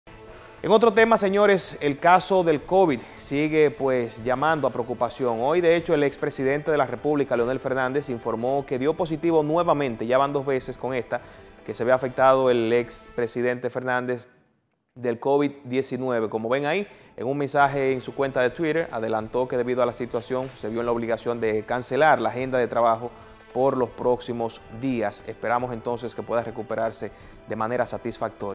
Leonel Fernández da positivo al COVID-19; suspende actividades
El expresidente de la República, Leonel Fernández informó este jueves que debido a que salió por segunda vez positivo al Covid-19, suspendió las actividades políticas de los próximos días.

Fernández, líder del partido Fuerza del Pueblo explicó que está siguiendo las indicaciones de los médicos y el debido aislamiento, al tiempo que llamó a la población a cuidarse de un rebrote del virus.